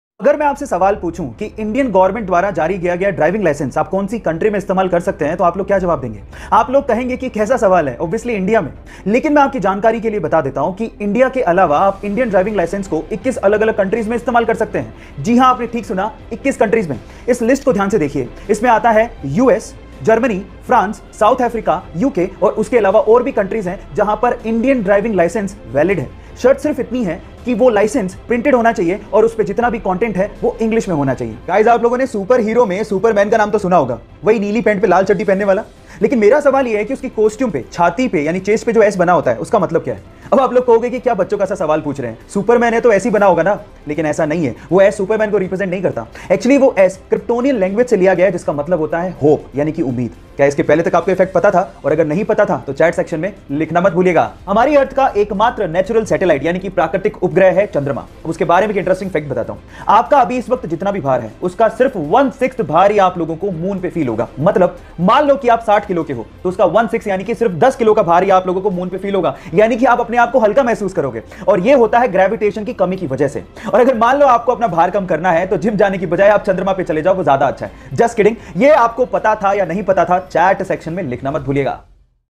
Driving Licence कितने देशों में Use कर सकते हैं?🤔 Amazing Facts 🔥
In how many countries can you use your driving licence?

This is fan page of Arvind Arora Sir...

Official Link of A2 Motivation ( Arvind Arora ) -